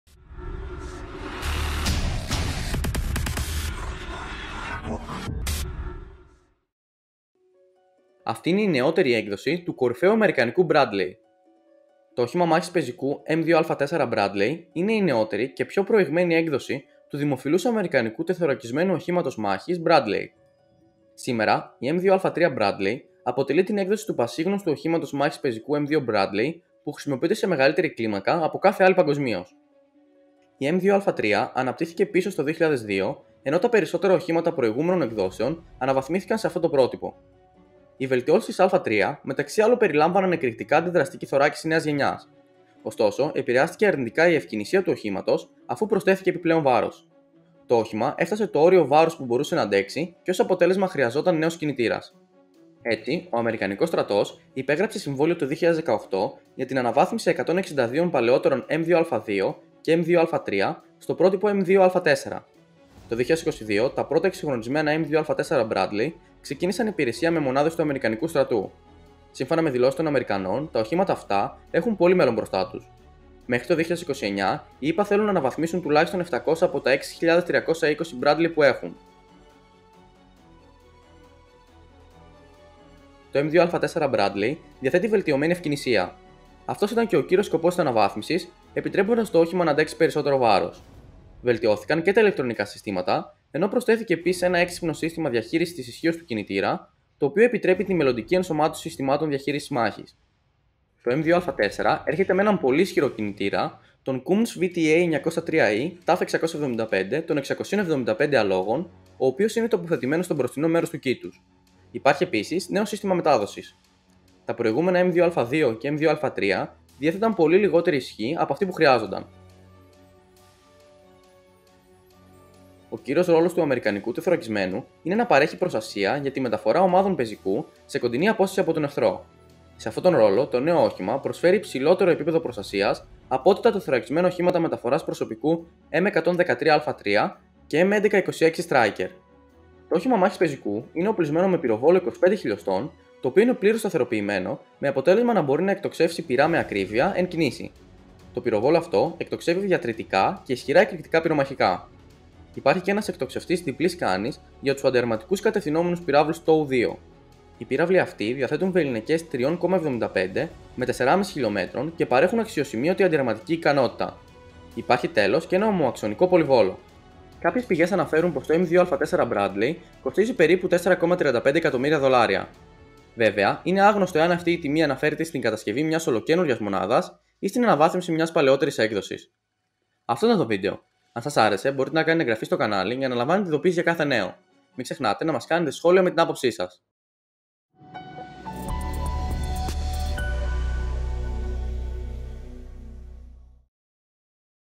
Η νέα έκδοση του Bradley ήρθε για να μείνει!
Σε αυτό το κανάλι ανακαλύπτουμε μαζί όλα τα στρατιωτικά και διεθνή νέα, σας ενημερώνουμε για τους νέους εξοπλισμούς και όλα τα κορυφαία όπλα σε παγκόσμιο επίπεδο.

Κεφάλαια:
00:00 - Εισαγωγή
01:19 - Νέες Βελτιώσεις
01:59 - Ρόλος και Οπλισμός

Το όχημα μάχης πεζικού M2A4 Bradley είναι η νεότερη και πιο προηγμένη έκδοση του δημοφιλούς αμερικανικού ΤΟΜA Bradley
Σήμερα η M2A3 Bradley αποτελεί την έκδοση του πασίγνωστου οχήματα μάχης πεζικού M2 Bradley που χρησιμοποιείται σε μεγαλύτερη κλίμακα από κάθε άλλη παγκοσμίως. Η M2A3 αναπτύχθηκε πίσω στο 2002, ενώ τα περισσότερα οχήματα προηγούμενων εκδόσεων αναβαθμίστηκαν σε αυτό το πρότυπο. Οι βελτιώσεις της "A3" μεταξύ άλλων περιλάμβαναν εκρηκτικά αντιδραστική θωράκιση νέας γενιάς. Ωστόσο, επηρεάστηκε αρνητικά η ευκινησία του οχήματος αφού προστέθηκε επιπλέον βάρος. Το όχημα έφτασε το όριο βάρους που μπορούσε να αντέξει και ως αποτέλεσμα χρειαζόταν νέος κινητήρας.

Έτσι, ο αμερικανικός στρατός υπέγραψε συμβόλαιο το 2018 για την αναβάθμιση 162 παλαιότερων M2A2 και M2A3 στο πρότυπο M2A4. Το 2022 τα πρώτα εκσυγχρονισμένα M2A4 Bradley ξεκίνησαν υπηρεσία με μονάδες του αμερικανικού στρατού. Σύμφωνα με δηλώσεις των Αμερικανών, τα οχήματα αυτά έχουν πολύ μέλλον μπροστά τους. Μέχρι το 2029 οι ΗΠΑ θέλουν να αναβαθμίσουν τουλάχιστον 700 από τα 6.320 Bradley που έχουν.

Νέες Βελτιώσεις
Το M2A4 Bradley διαθέτει βελτιωμένη ευκινησία. Αυτός ήταν και ο κύριος σκοπός της αναβάθμισης, επιτρέποντας στο όχημα να αντέξει περισσότερο βάρος. Βελτιώθηκαν και τα ηλεκτρονικά συστήματα, ενώ προστέθηκε επίσης ένα έξυπνο σύστημα διαχείρισης της ισχύος του κινητήρα, το οποίο επιτρέπει την μελλοντική ενσωμάτωση συστημάτων διαχείρισης μάχης.

Το M2A4 έρχεται με έναν πολύ ισχυρό κινητήρα, τον Cummns VTA-903E-T675 των 675 αλόγων, ο οποίος είναι τοποθετημένος στο μπροστινό μέρος του κύτους. Υπάρχει επίσης νέο σύστημα μετάδοσης. Τα προηγούμενα M2A2 και M2A3 διέθεταν πολύ λιγότερη ισχύ από αυτή που χρειάζονταν.

Ρόλος και Οπλισμός
Ο κύριος ρόλος του αμερικανικού τεθωρακισμένου είναι να παρέχει προστασία για την μεταφορά ομάδων πεζικού σε κοντινή απόσταση από τον εχθρό. Σε αυτόν τον ρόλο, το νέο όχημα προσφέρει υψηλότερο επίπεδο προστασίας από ότι τα ΤΟΜΠ M113A3 και M1126 Stryker.

Το όχημα μάχης πεζικού είναι οπλισμένο με πυροβόλο 25 χιλιοστών, το οποίο είναι πλήρως σταθεροποιημένο με αποτέλεσμα να μπορεί να εκτοξεύσει πυρά με ακρίβεια εν κινήσει. Το πυροβόλο αυτό εκτοξεύει διατρητικά και ισχυρά εκρηκτικά πυρομαχικά. Υπάρχει και ένας εκτοξευτής διπλής κάννης για τους αντιαρματικούς κατευθυνόμενους πυραύλους TOW 2. Οι πύραυλοι αυτοί διαθέτουν βεληνεκές 3,75 με 4,5 χιλιομέτρων και παρέχουν αξιοσημείωτη αντιαρματική ικανότητα. Υπάρχει τέλος και ένα ομοαξονικό πολυβόλο.

Κάποιες πηγές αναφέρουν πως το M2A4 Bradley κοστίζει περίπου 4,35 εκατομμύρια δολάρια. Βέβαια, είναι άγνωστο εάν αυτή η τιμή αναφέρεται στην κατασκευή μίας ολοκαίνουριας μονάδας ή στην αναβάθμιση μίας παλαιότερης έκδοσης.